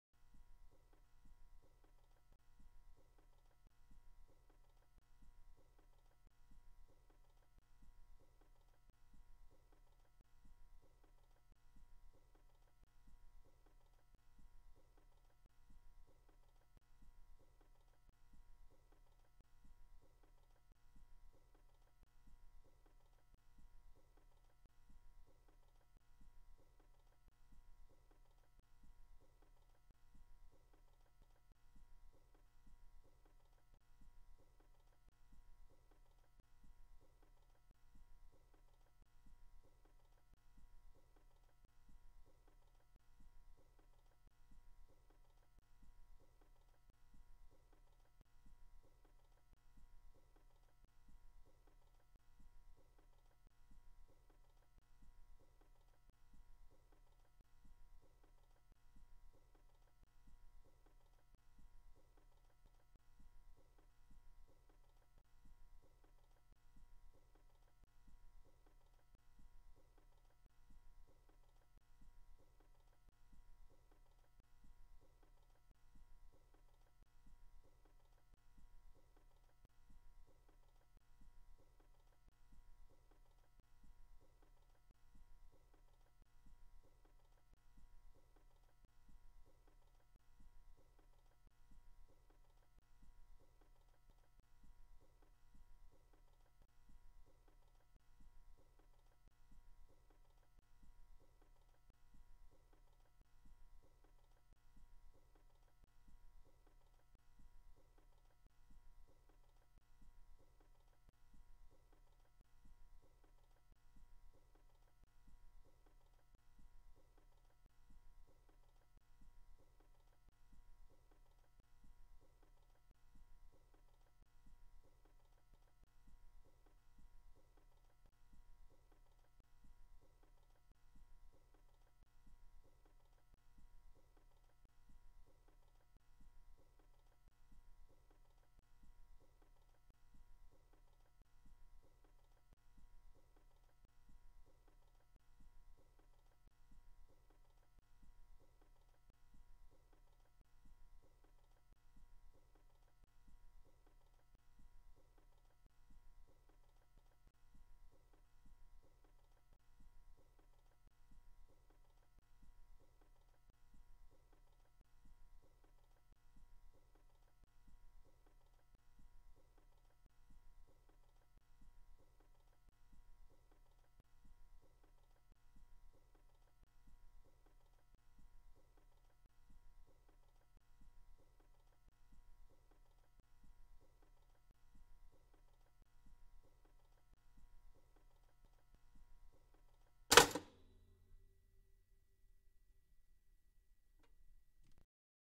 Non Relaxing Sounds - Anxiety Waiting For The Toaster To Pop
Non Relaxing Sounds for your anxiety. Stay awake waiting for these exciting events to happen.